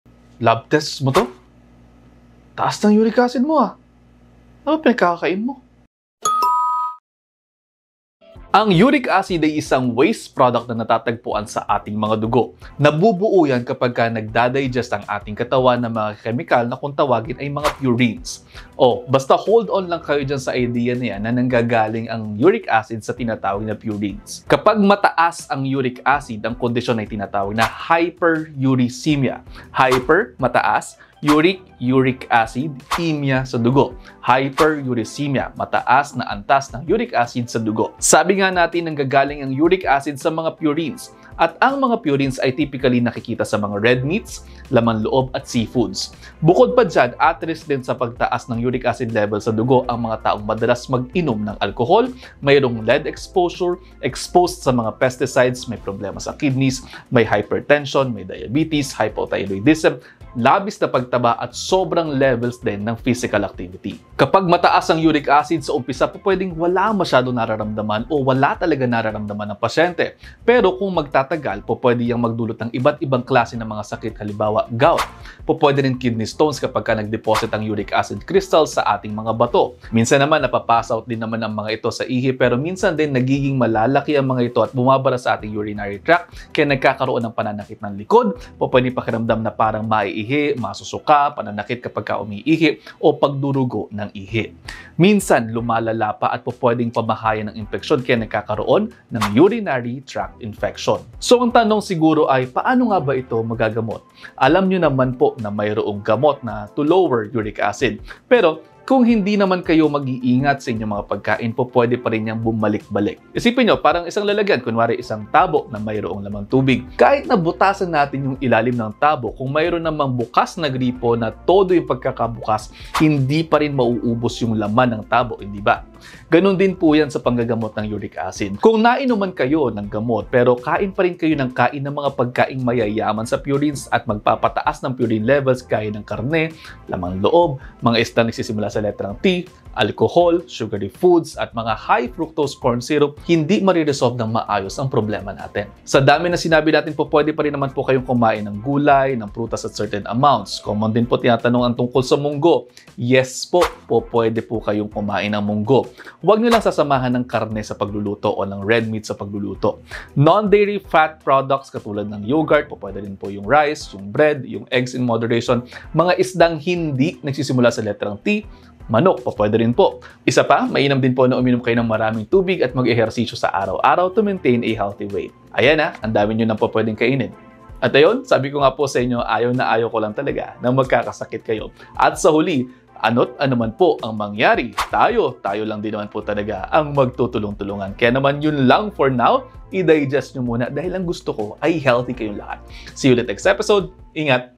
Uric Acid mataas, ano kakainin?
Uric Acid mataas, ano kakainin? Panoorin mo 'to!

Doctor Dex here, I am your Real Nutrition Doctor.